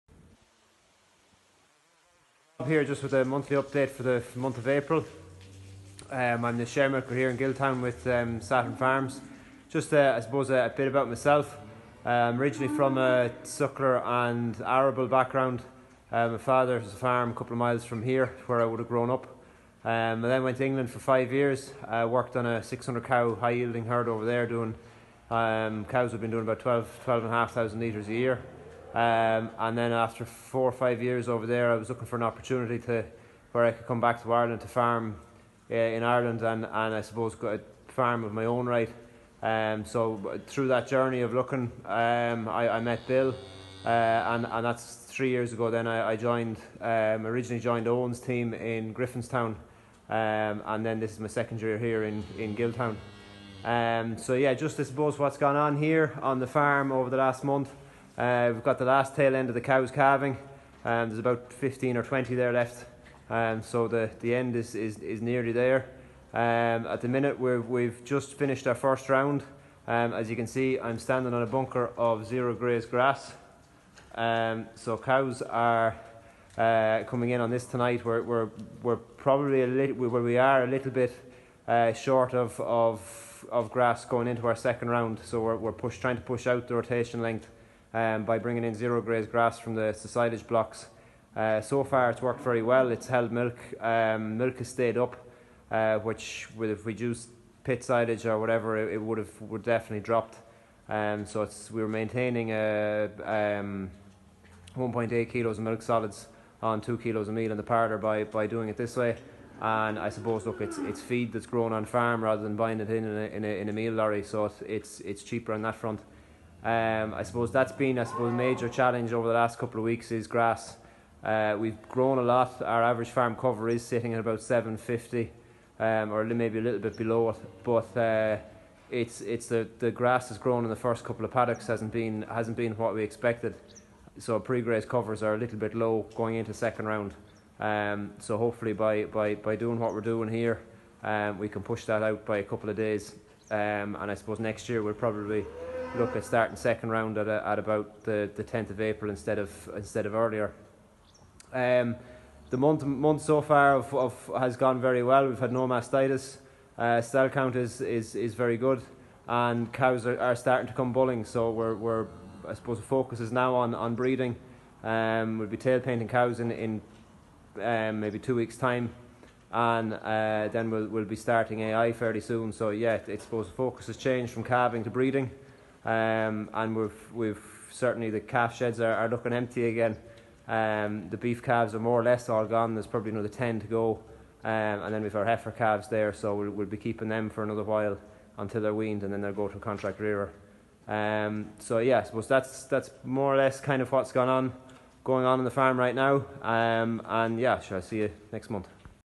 Sharemilking Saturn Farms Rob Colton April week 1 2019
sharemilking Sharemilker Rob Colton is from a suckler and arable background. The family farm is only a couple miles away from Gilltown farm. After college Rob moved to the UK to work on a 600 cow high yielding dairy farm. Cows were doing an average of 12,500 litres. Rob wanted to farm in his own right when coming back to Ireland and made the decision to join Saturn Farms. Originally Rob joined Eoghan O Dwyers team in Griffinstown. Rob is now in his second year in Gilltown farming in his own right.
On the farm there is around 15 cows left to calve. Rob has just finished first round. Cows are on zero graze grass to slow down the second round. It's working well as cows milking well at 1.8kgs MS for a herd of second calvers.Average farm cover is at 750kg's of dry matter.Next year Rob will shove out first rotation end date to 10th of April.
There has been no mastitis in last month which he is happy with.Cows are bulling away and tail painting will start in 2 weeks time. The total focus is now turning to breeding.
Calf sheds are now nearly empty and only heifers calves left on farm. These will go to the contract rearers in May.